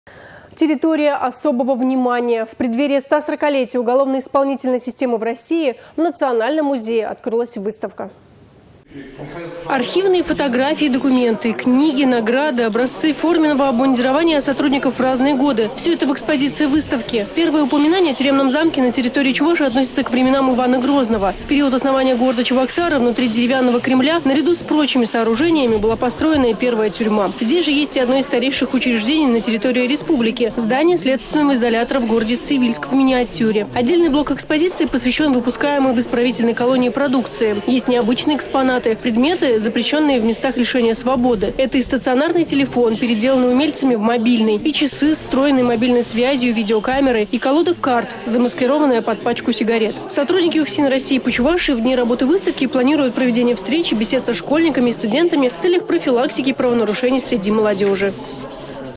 «Территория особого внимания»
В преддверии 140-летия уголовно-исполнительной системы в России в  Национальном музее открылась выставка.